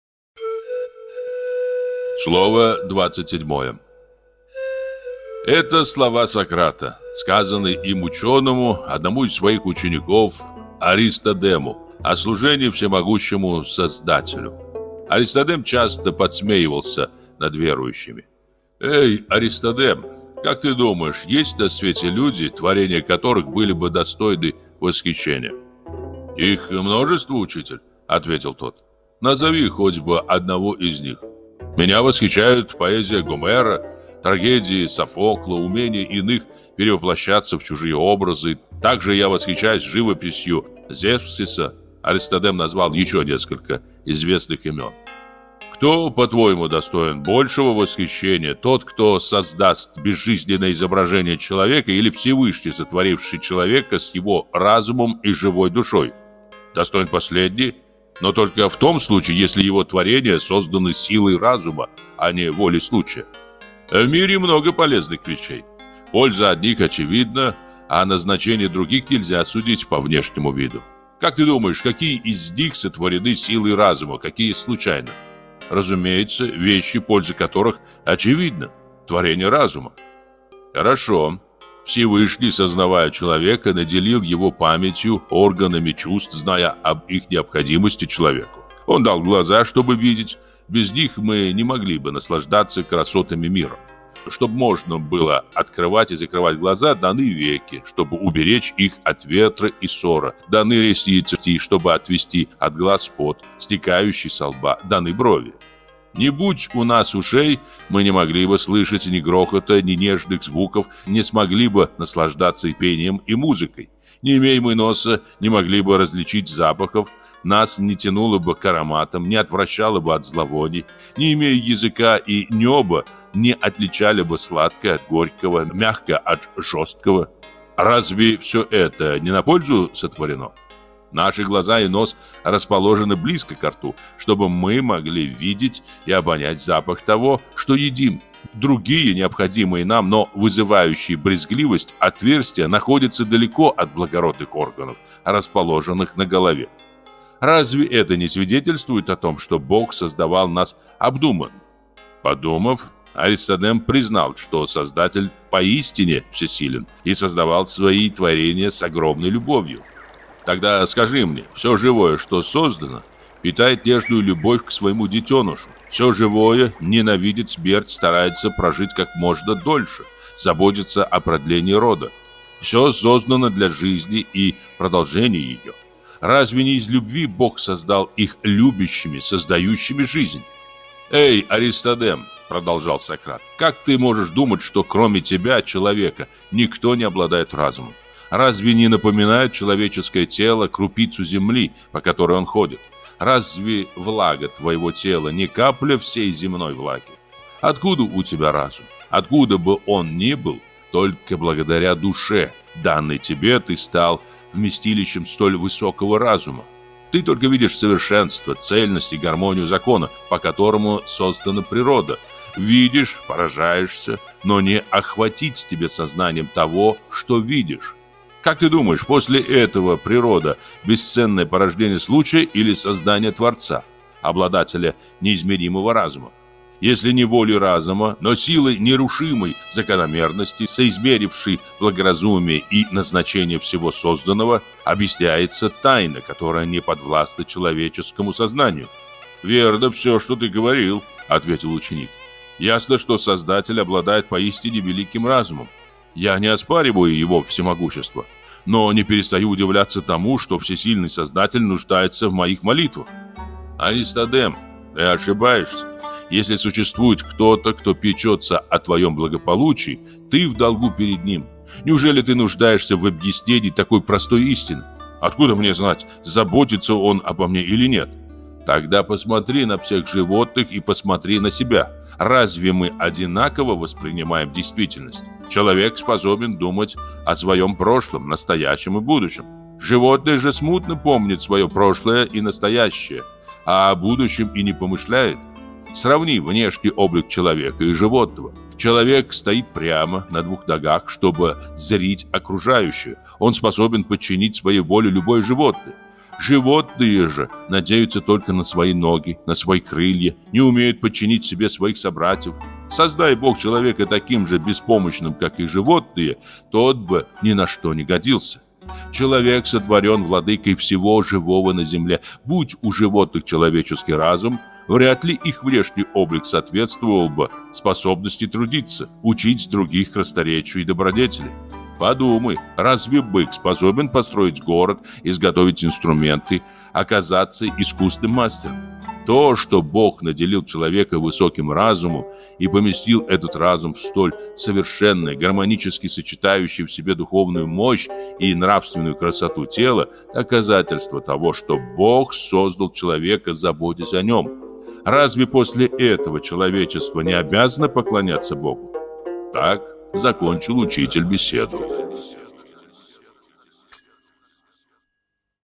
Абай Кунанбаев: Слова назидания. Слово двадцать седьмое.
Это слова Сократа, сказанные им ученому, одному из своих учеников Аристодему о служении всемогущему Создателю. Аристодем часто подсмеивался над верующими.

- Эй, Аристодем, как ты думаешь, есть на свете люди, творения которых были бы достойны восхищения?

- Их множество, учитель,- ответил тот.

- Назови хотя бы одного из них.

- Меня восхищают поэзия Гомера, трагедии Софокла, умение иных перевоплощаться в чужие образы, так же я восхищаюсь живописью Зевксиса,- Аристодем назвал еще несколько известных имен.

- Кто по-твоему достоин большего восхищения, тот, кто создаст безжизненное изображение человека или Всевышний, сотворивший человека с его разумом и живой душой?

- Достоин последний. Но только в том случае, если его творения созданы силой разума, а не волей случая.

- В мире много полезных вещей. Польза одних очевидна, о назначении других нельзя судить по внешнему виду. Как ты думаешь, какие из них сотворены силой разума, какие случайно?

- Разумеется, вещи, польза которых очевидна — творения разума.

- Хорошо. Всевышний, создавая человека, наделил его пятью органами чувств, зная об их необходимости человеку. Он дал глаза, чтоб видеть, без них мы не могли бы наслаждаться красотами мира. Чтоб можно было открывать и закрывать глаза, даны веки, чтоб уберечь их от ветра и сора, даны ресницы, чтоб отвести от глаз пот, стекающий со лба, даны брови.

Не будь у нас ушей, мы не могли бы слышать ни грохота, ни нежных звуков, не смогли бы наслаждаться пением и музыкой. Не имей мы носа, не могли бы различить запахов, нас не тянуло бы к ароматам, не отвращало бы от зловоний. Не имея языка и неба, не отличали бы сладкое от горького, мягкое от жесткого.

Разве все это не на пользу сотворено?

Наши глаза и нос расположены близко ко рту, чтобы мы могли видеть и обонять запах того, что едим. Другие необходимые нам, но вызывающие брезгливость, отверстия находятся далеко от благородных органов, расположенных на голове.

Разве это не свидетельствует о том, что Бог создавал нас обдуманно?

Подумав, Аристодем признал, что Создатель поистине всесилен, и создавал свои творения с огромной любовью.

- Тогда скажи мне, все живое, что создано, питает нежную любовь к своему детенышу, все живое ненавидит смерть, старается прожить как можно дольше, заботится о продлении рода. Все создано для жизни и продолжения ее. Разве не из любви Бог создал их любящими, созидающими жизнь?

- Эй, Аристодем,- продолжал Сократ,- как ты можешь думать, что кроме тебя, человека, никто не обладает разумом? Разве не напоминает человеческое тело крупицу земли, по которой он ходит? Разве влага твоего тела не капля всей земной влаги? Откуда у тебя разум? Откуда бы он ни был, только благодаря душе, данной тебе, ты стал вместилищем столь высокого разума. Ты только видишь совершенство, цельность и гармонию закона, по которому создана природа, видишь, поражаешься, но не охватить тебе сознанием того, что видишь.

Как ты думаешь после этого, природа — бесцельное порождение случая или создание творца — обладателя неизмеримого разума? Если не волей разума, то силой нерушимой закономерности, соизмерившей благоразумие и назначение всего созданного, объясняется тайна, которая не подвластна человеческому сознанию.

- Верно все, что ты говорил,- ответил ученик,- ясно, что Создатель обладает поистине великим разумом. Я не оспариваю его всемогущества. Но не перестаю удивляться тому, что всесильный Создатель нуждается в моих молитвах.

- Аристодем! Ты ошибаешься. Если существует кто-то, кто печется о твоем благополучии — ты в долгу перед ним. Неужели ты нуждаешься в объяснении такой простой истины?

- Откуда мне знать, заботится он обо мне или нет?

- Тогда посмотри на всех животных и посмотри на себя. Разве мы одинаково воспринимаем действительность? Человек способен думать о своем прошлом, настоящем и будущем. Животное же смутно помнит свое прошлое и настоящее, а о будущем и не помышляет. Сравни внешний облик человека и животного. Человек стоит прямо, на двух ногах, чтобы зрить окружающее. Он способен подчинить своей воле любое животное. Животныеже надеются только на свои ноги, на свои крылья, не умеют подчинить себе своих собратьев. Создай Бог человека таким же беспомощным, как животные, тот бы ни на что не годился. Человек сотворен владыкой всего живого на земле. Будь у животных человеческий разум, вряд ли их внешний облик соответствовал бы способности трудиться, учить других красноречию и добродетели. Подумай, разве бык способен построить город, изготовить инструменты, оказаться искусным мастером? То, что Бог наделил человека высоким разумом и поместил этот разум в столь совершенное, гармонически сочетающее в себе духовную мощь и нравственную красоту тело, доказательство того, что Бог создал человека, заботясь о нем. Разве после этого человечество не обязано поклоняться Богу?

Так закончил учитель беседу.